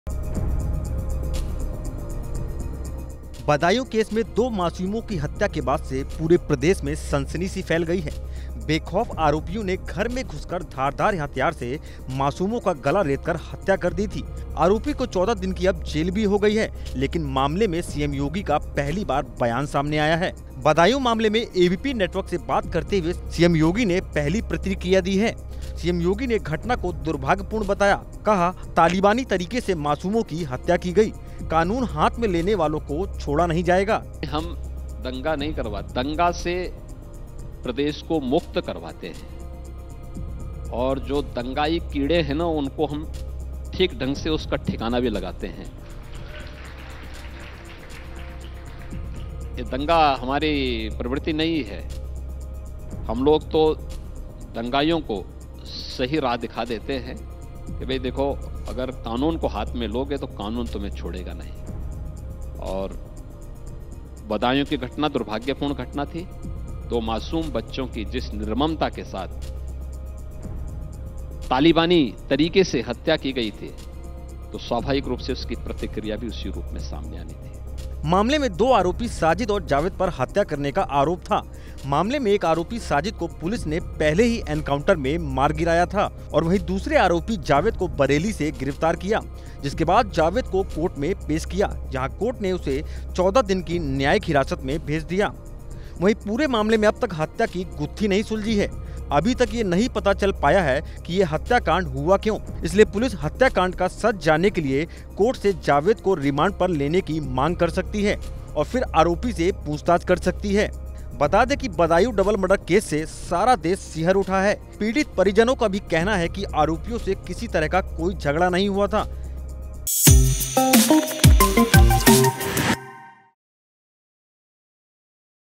Badaun Case: CM Yogi की बड़ी चेतावनी- कानून को हाथ में लेने वालों को बख्शा नहीं जाएगा | BJP
यूपी के बदायूं जिले में मंगलवार को दो मासूम बच्चों की मौत से सनसनी फैल गई जिसके बाद पुलिस एनकाउंटर में हत्या का आरोपी साजिद मारा गया. अब इस मर्डर केस में सीएम योगी ने पहली बार प्रतिक्रिया दी है. सीएम ने कहा है कि कानून को हाथ में लेने वालों को बख्शा नहीं जाएगा.